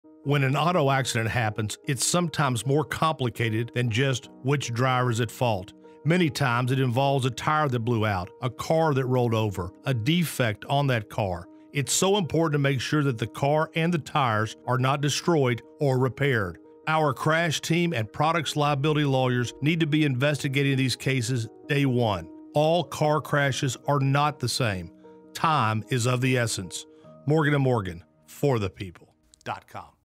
All Crashes Are Not the Same | Attorney John Morgan
When an auto accident happens, it’s sometimes more complicated than just which driver is at fault.

Many times it involves a tire that blew out, a car that rolled over, a defect on that car - it’s so important to make sure that the car and the tires are not destroyed or repaired.

Our Crash Team and Products Liability lawyers need to be investigating these cases - day one.

All car crashes are not the same. Time is of the essence.

Morgan & Morgan
ForThePeople.com